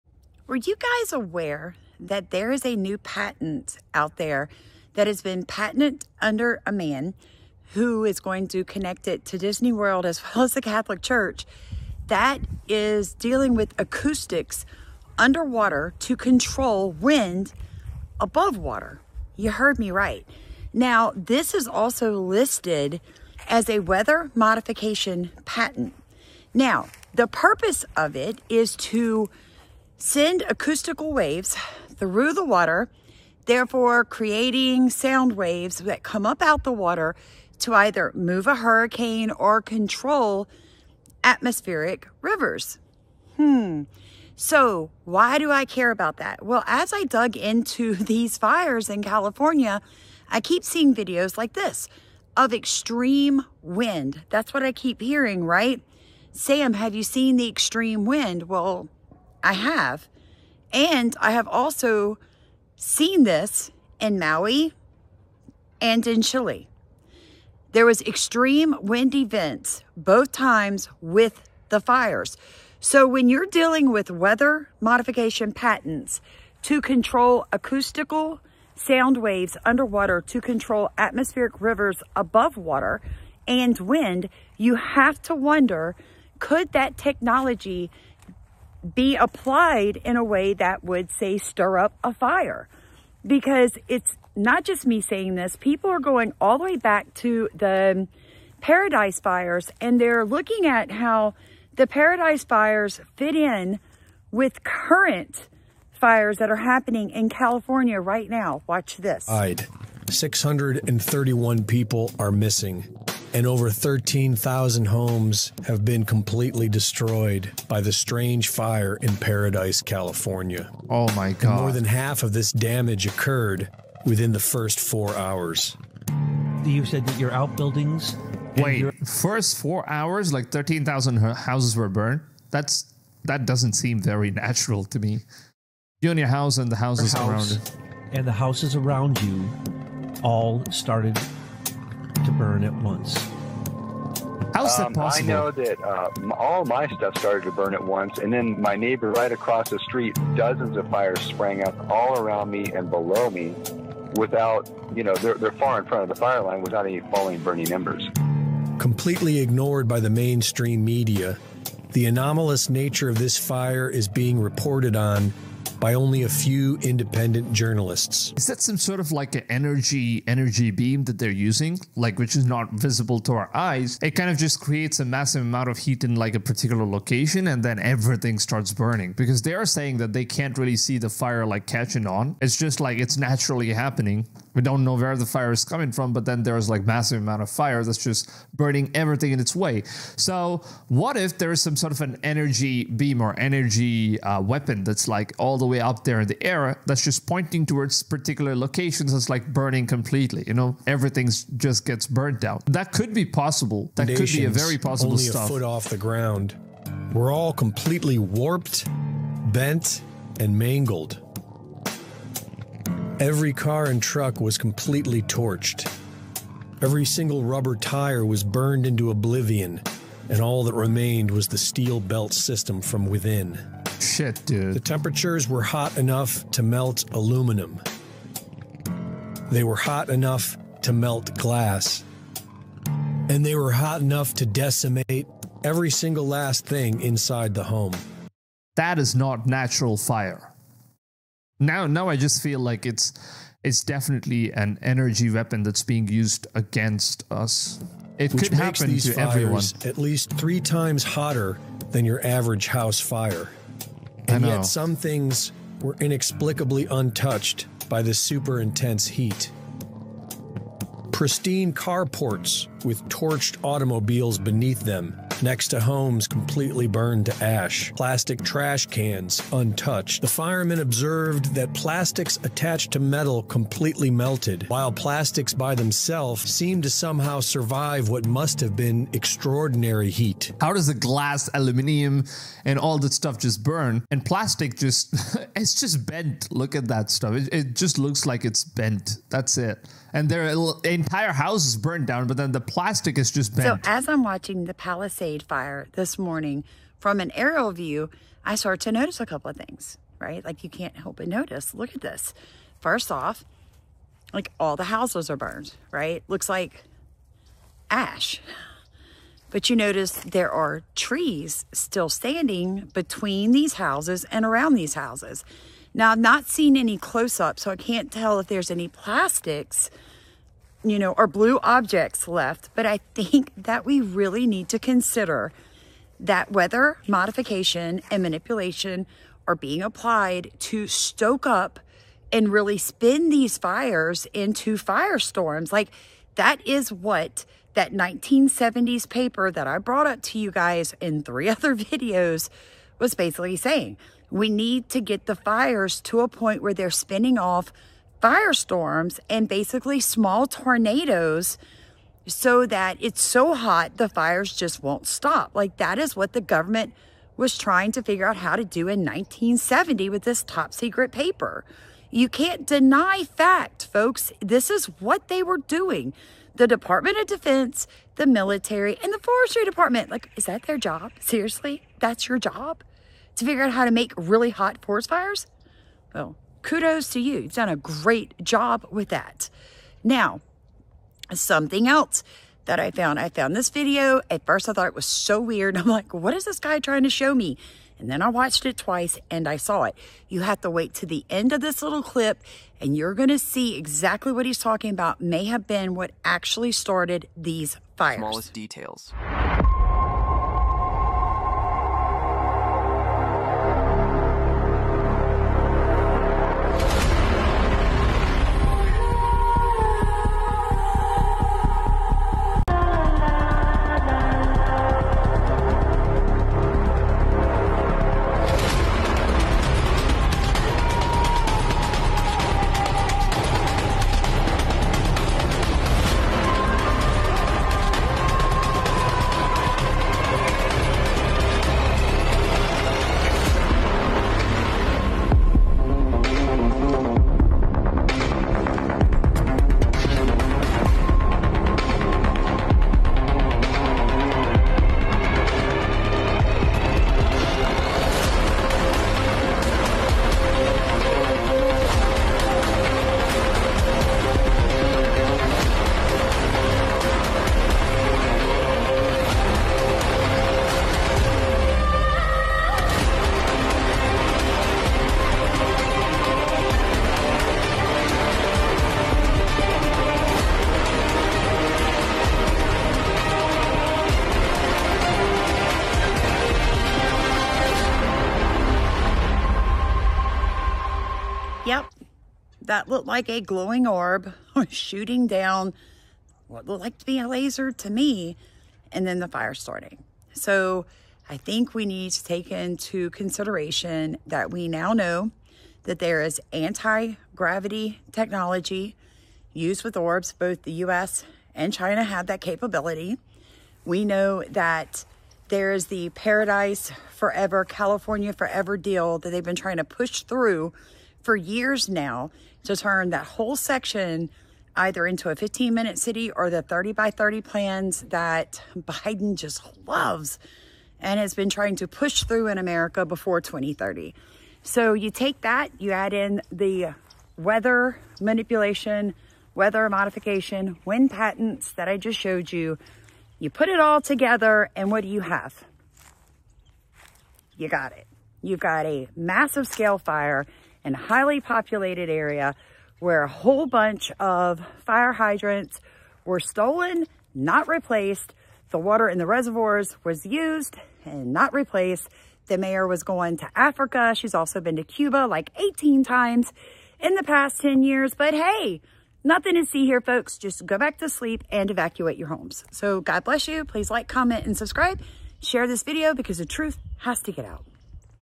EXTREME WIND EVENTS??? wind patents vs California fires
A lot of these clips come from ticktock and instagram.

Weather manipulation
• US11632916B2: This patent covers a process for propagating soundwaves through water to create and direct winds. This process could be used to weaken storms and guide atmospheric rivers.

Another purpose is to guide atmospheric rivers, and manually re-direct clouds in the precipitation cycle. This inventive process grants methods to mitigate dangerous weather patterns, such as droughts and hurricanes. The invention introduces a new subject matter that distinguishes it from other inventions relevant to underwater acoustics: manual processes to moderate weather.

Sound causes water to move in the direction the sound is being propagated. The propagation of sound in water causes motion, forming compressions and rarefactions through the liquid. These waves are capable of shifting pockets of air between their peaks and troughs.
Additionally, as sound is propagated through the liquid body of water, additional water evaporates, inheriting momentum from the propagation of sound, and creating a larger gust of wind to affect the weather.
The transfer of the sound's momentum through the water is sufficient to direct wind shear powerful enough to weaken storms, guide clouds and precipitation, and affect various other weather patterns.

ORBS above the fires, the billionaires that took California's water: CALIFORNIA ON FIRE

Eyesoftomorrow ticktock for orb video